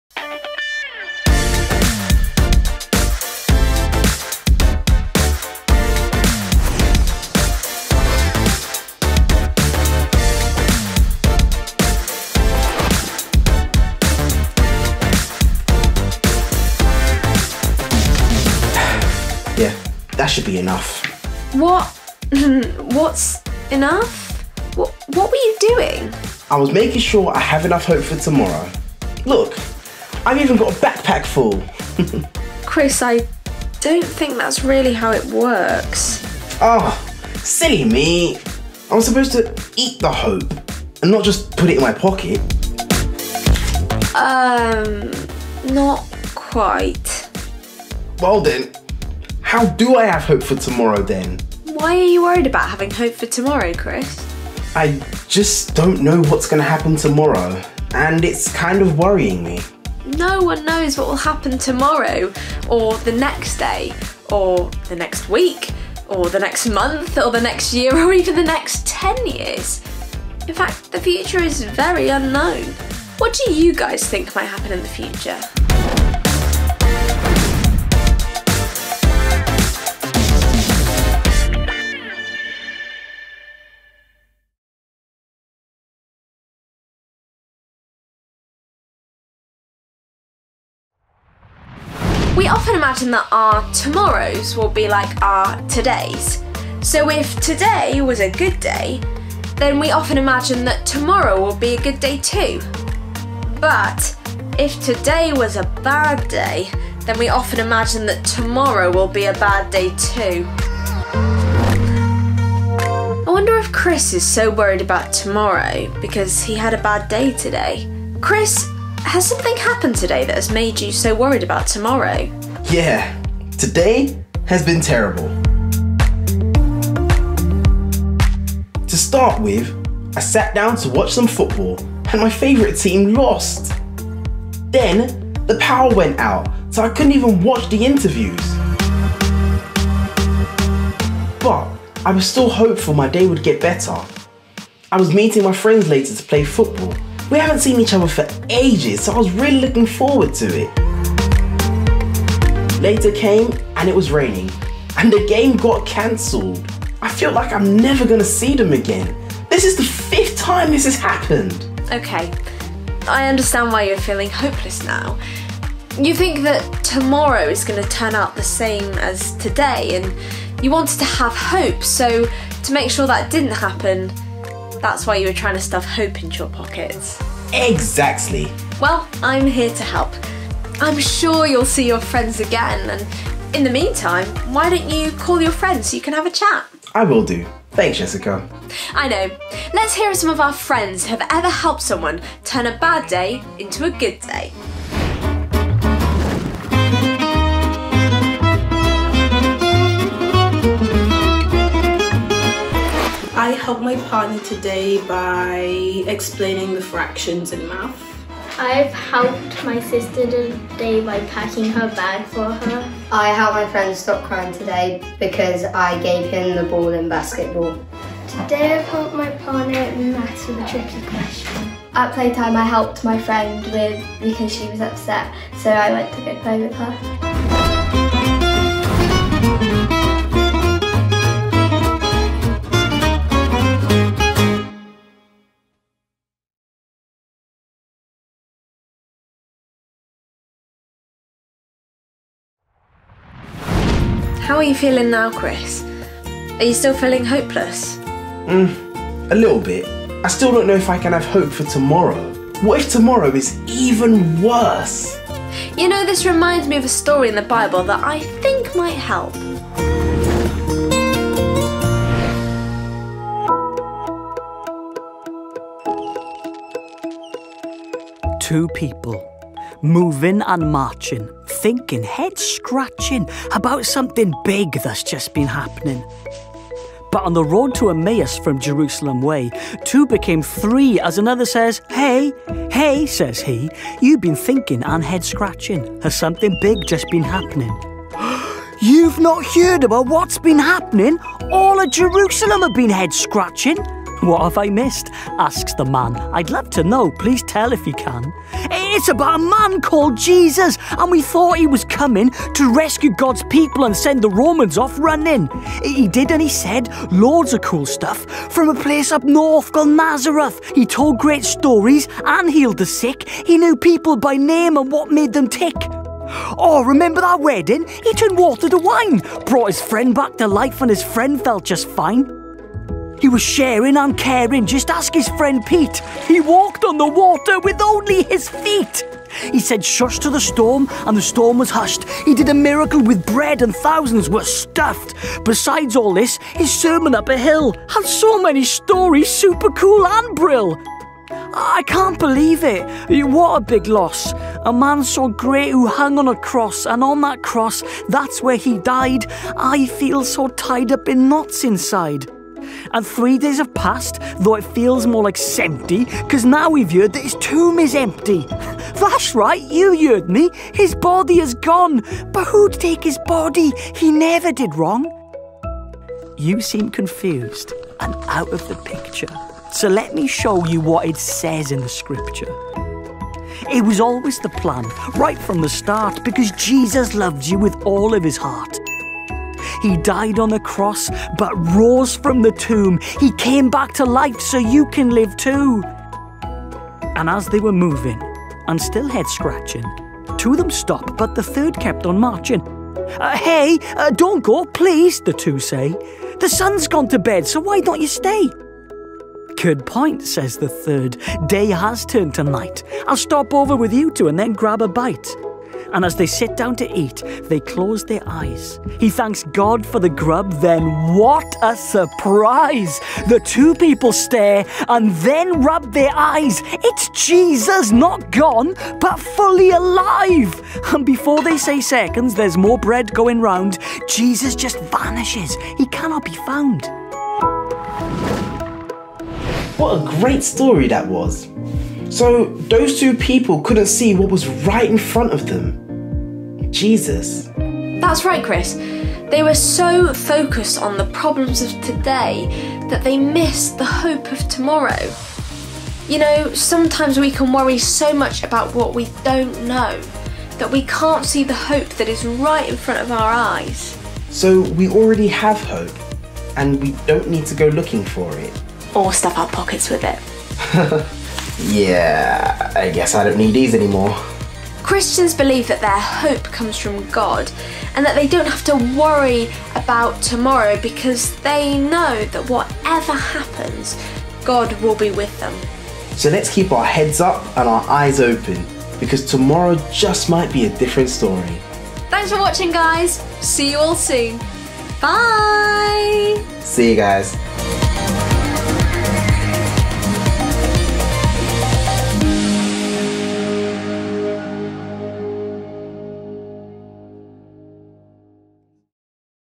Faith At Home for Primary Schools Collective Worship: Series 2 Episode 6 - Hope
Our sixth FaithAtHome Collective Worship episode for primary schools asks 'how can we have hope for tomorrow?'